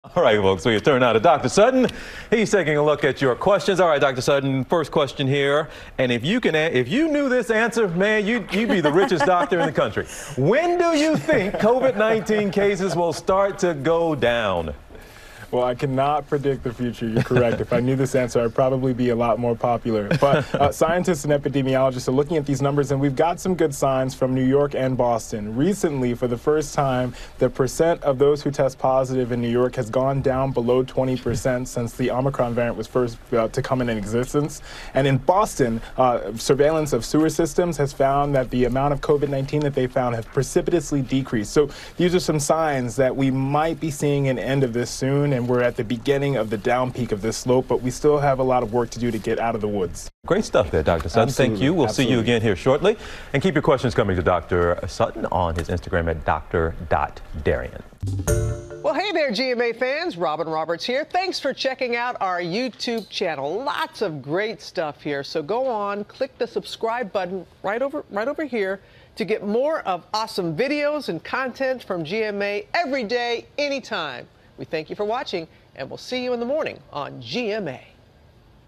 ‘When will COVID-19 cases will start to go down?’
Dr. Darien Sutton answers viewers’ health and COVID-19 questions.